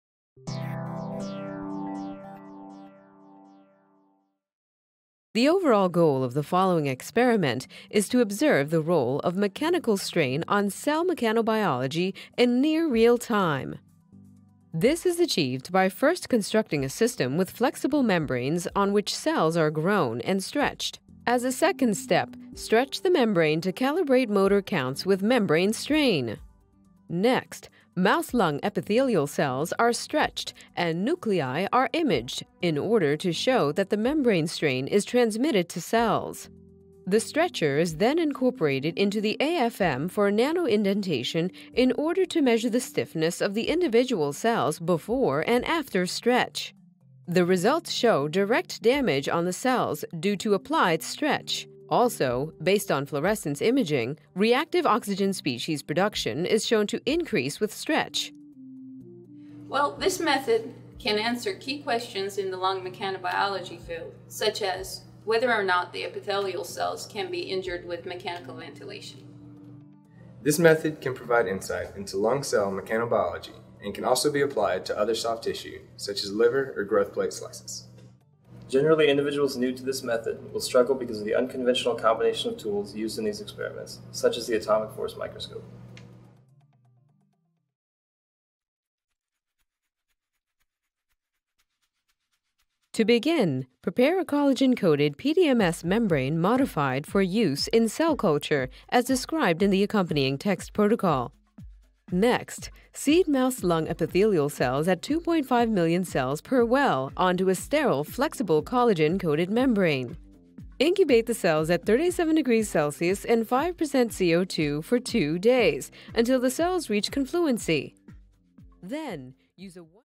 Mechanical Stretch: Live Cell Imaging
Live Cell Imaging during Mechanical Stretch -  a 2 minute Preview of the Experimental Protocol

Gabriel Rápalo, Josh D. Herwig, Robert Hewitt, Kristina R. Wilhelm, Christopher M. Waters, Esra Roan
University of Tennessee Health Science Center, Department of Physiology; University of Tennessee Health Science Center, Department of Biomedical Engineering and Imaging; University of Memphis, Department of Biomedical Engineering; University of Memphis, Department of Engineering Technology;

A novel imaging protocol was developed using a custom motor-driven mechanical actuator to allow the measurement of real time responses to mechanical strain in live cells. Relevant to mechanobiology, the system can apply strains up to 20% while allowing near real-time imaging with confocal or atomic force microscopy.